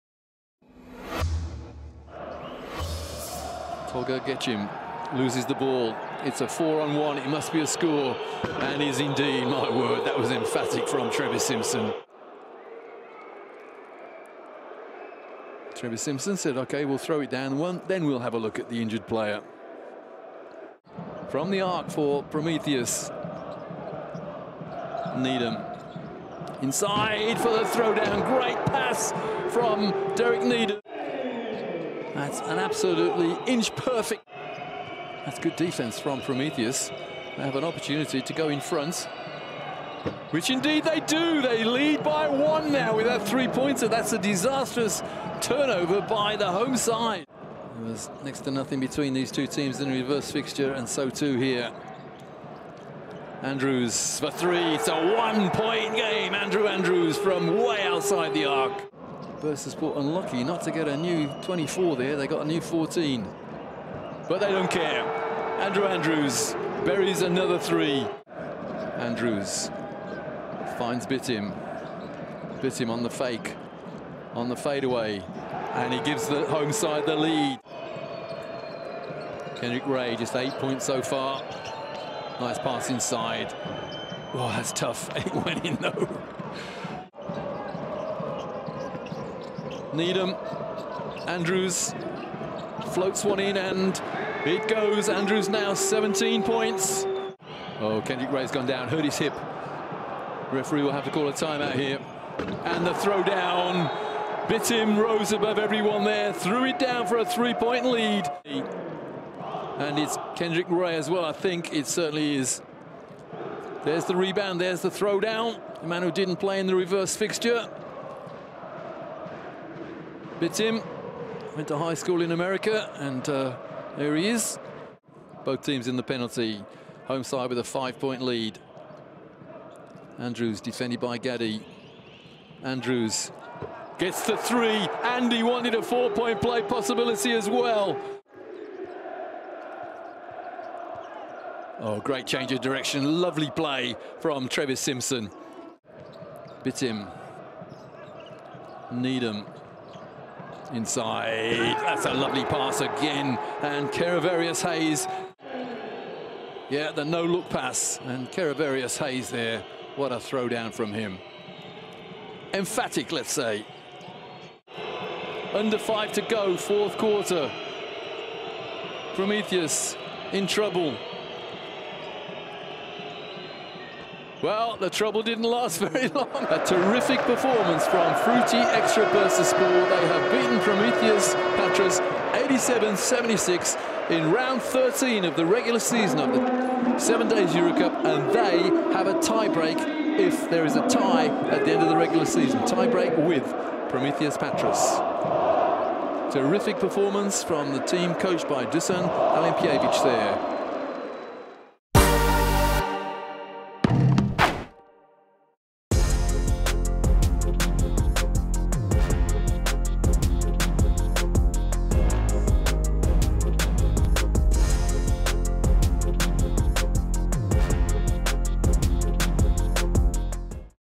Andrews leads Bursaspor past Promitheas! | Round 13, Highlights | 7DAYS EuroCup
In a key battle to reach the knock-out rounds, Frutti Extra Bursaspor broke its three-game losing streak in Group B by downing Promitheas Patras 87-76 at Tofas Spor Salonu on Tuesday. Bursaspor improved to 4-7 in the standings while Promitheas dropped to 3-9. Andrew Andrews led the winners with 24 points. Kevarrius Hayes added 17, Derek Needham 14, Onuralp Bitim 12 and Metin Turen 10 for Bursaspor. Dimitris Agravanis led Promitheas with 19 points. Kendrick Ray added 16 while Nikolaos Gkikas got 12 for the guests.